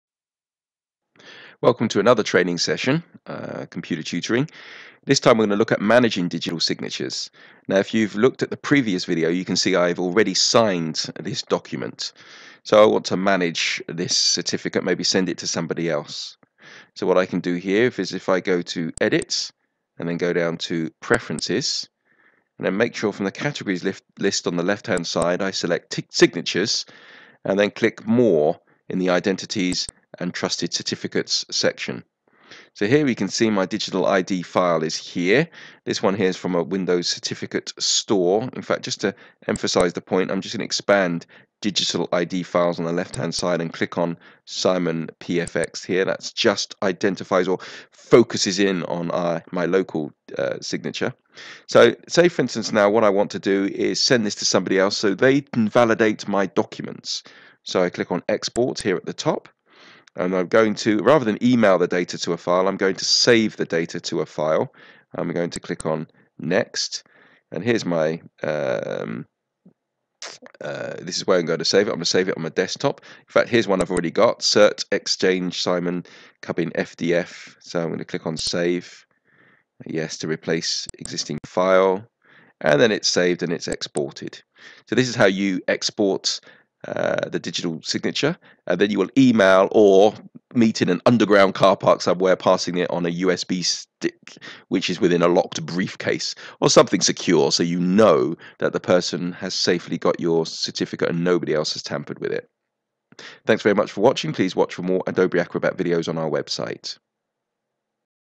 Exporting Digital Signatures in Adobe Acrobat XI Pro
Shows you how to export digital ID's in Acrobat so that someone else can import your trusted identity into another file.